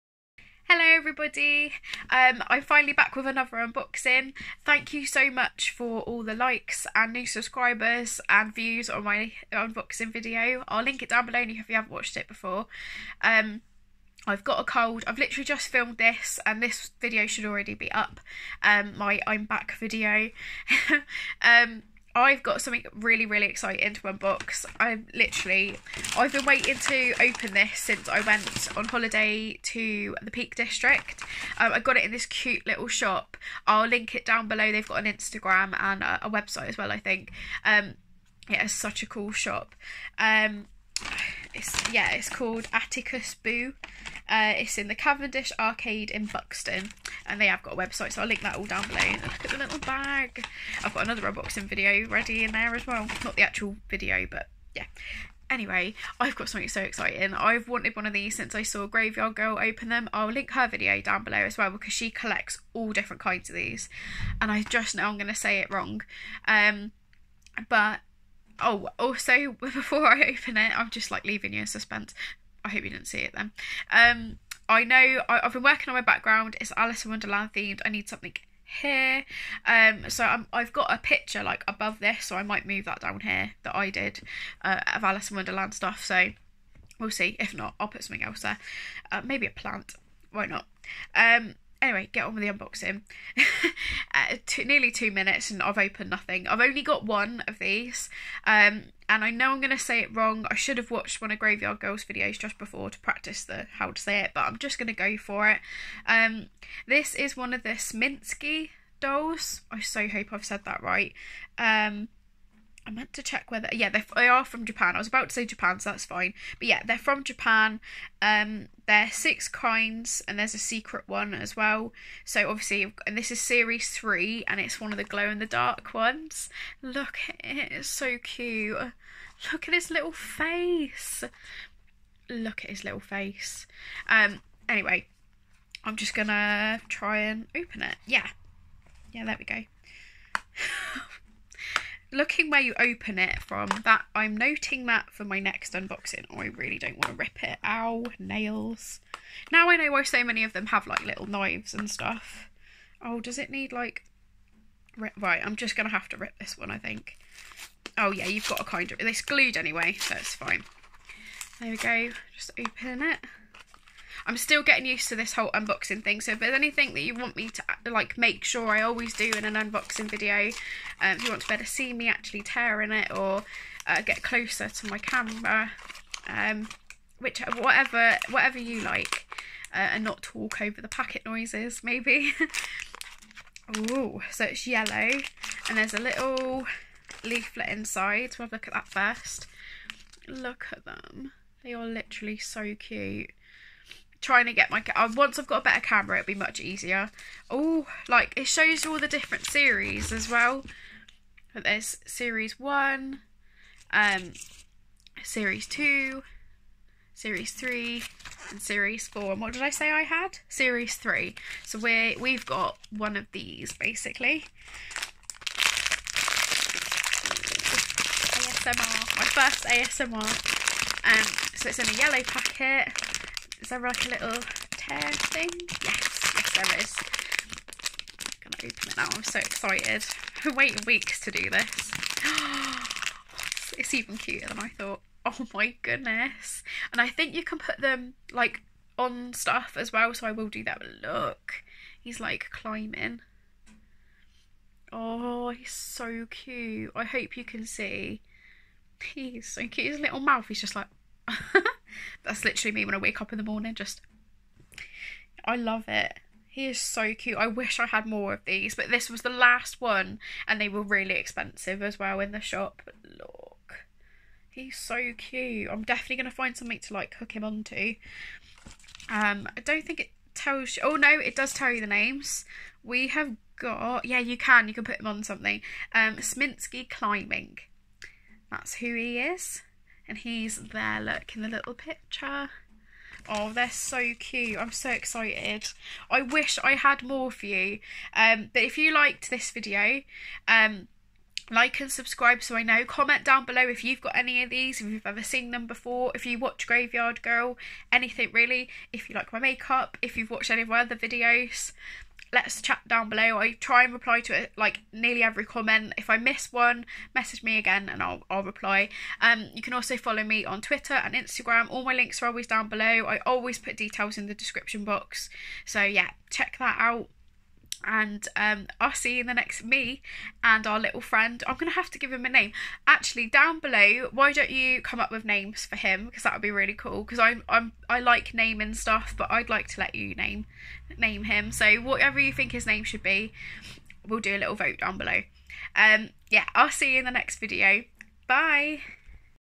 UNBOXING MY FIRST SMISKI DOLL!
I'm so excited to be back with another unboxing finally!

If you would like to use my link to try Glossybox for yourself we can both earn a discount☺️